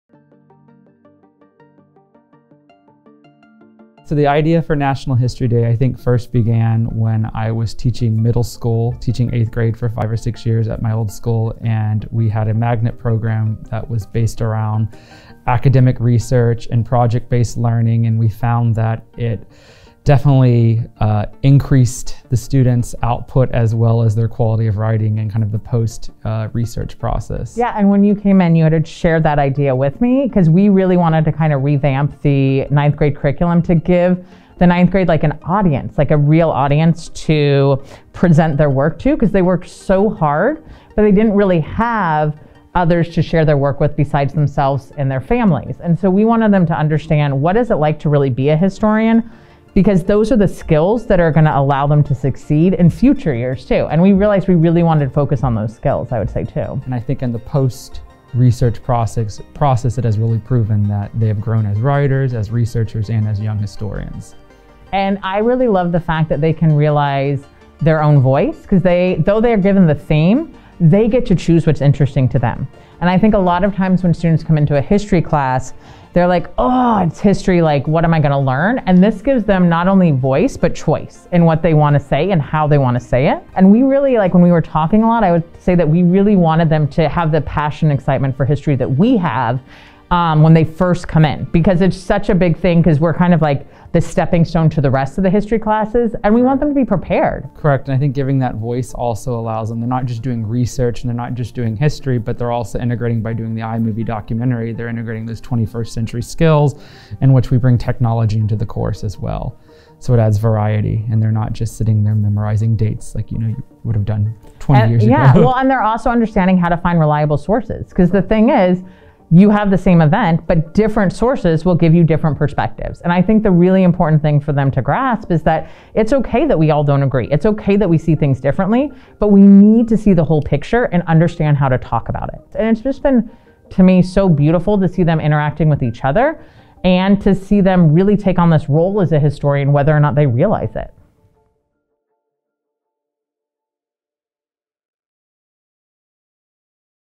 US History Fair
At Porter-Gaud, the 9th grade National History Day curriculum is a rigorous introduction to the research process. This project allows students to develop their historical voice and understand how to articulate an informed opinion through the analysis of evidence in the appropriate context. The National History Day contest encourages more than half a million students around the world to conduct historical research on a topic of their choice based on an annual theme.